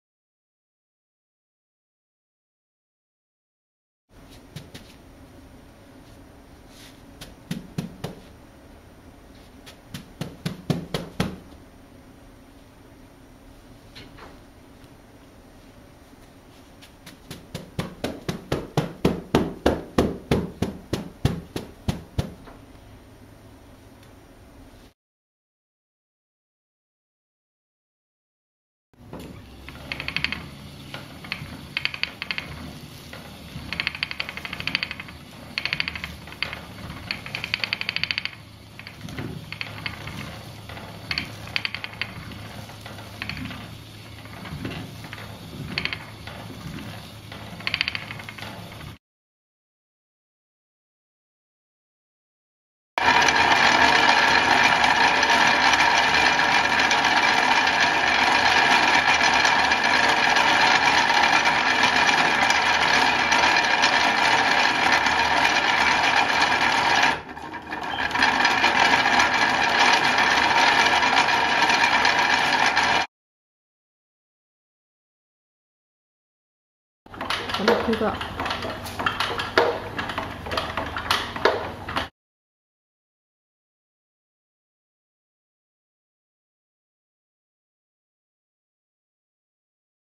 Hand spinning floss silk/真綿 紡ぎ
Hand spinning floss silk into silk yarn.
真綿の糸紡ぎ